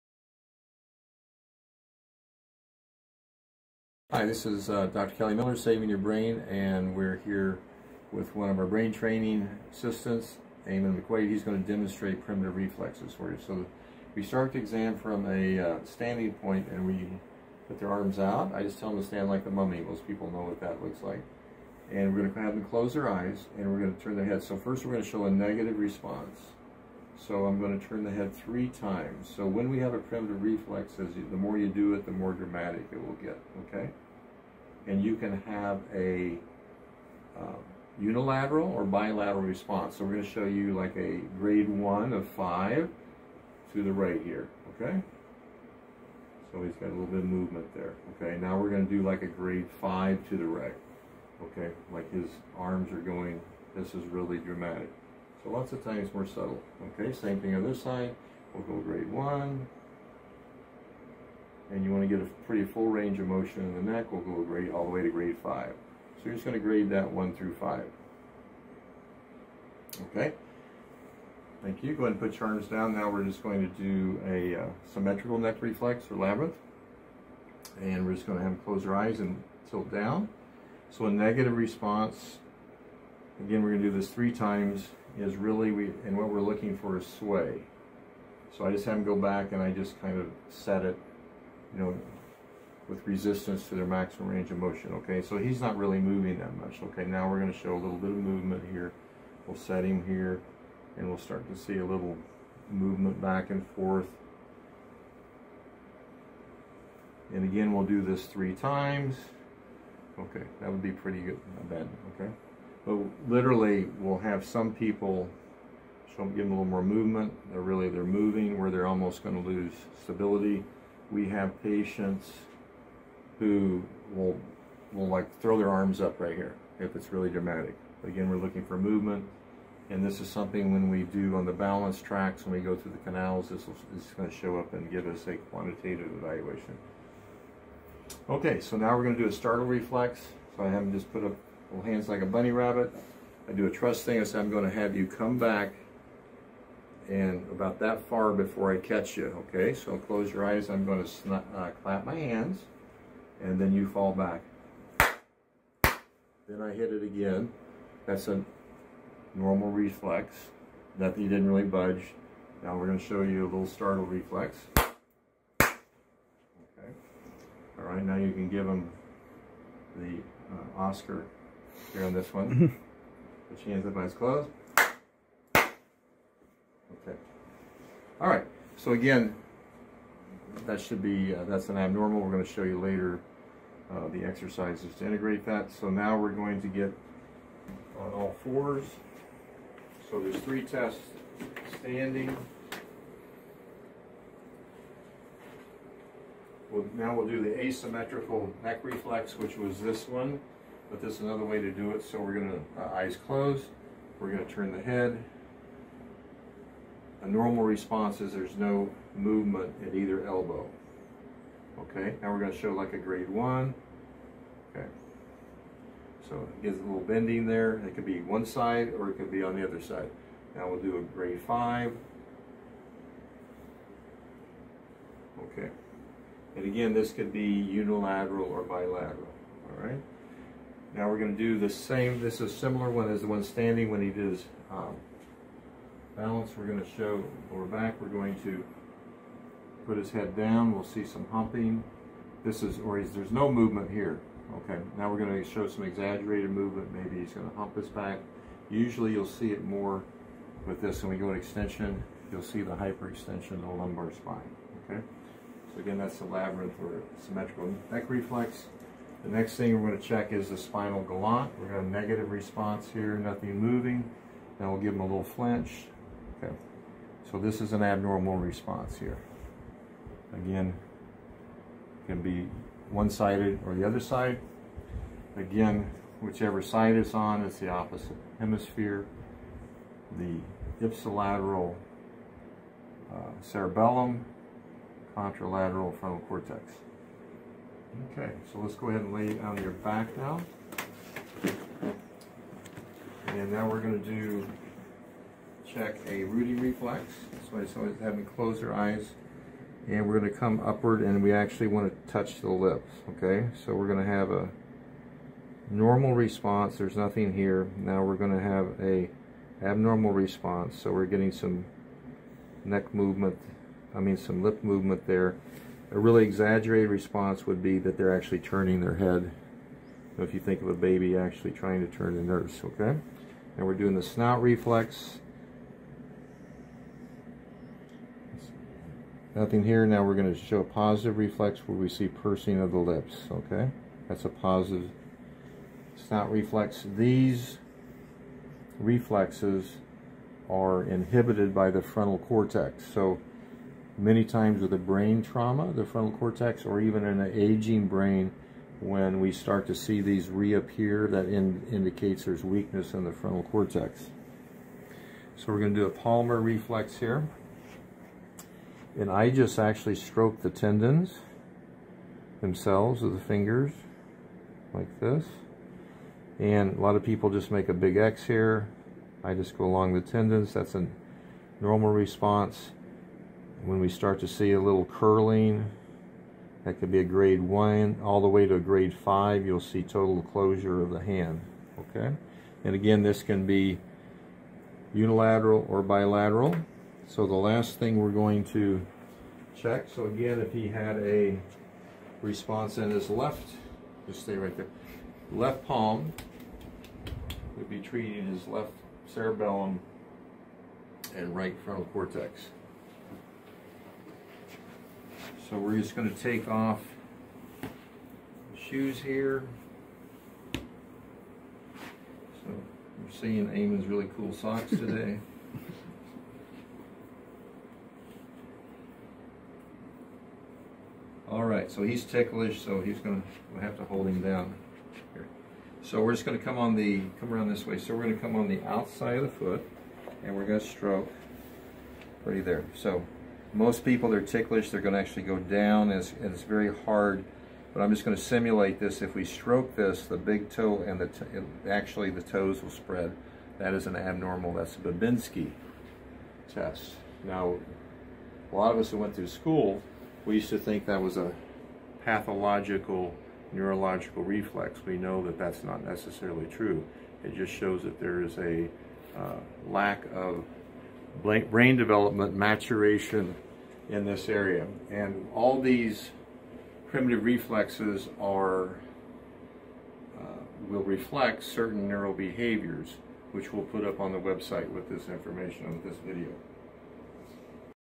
Primitive Reflex Testing
Kelly Miller, NMD performs Primitive Reflex examination at Saving Your Brain centers.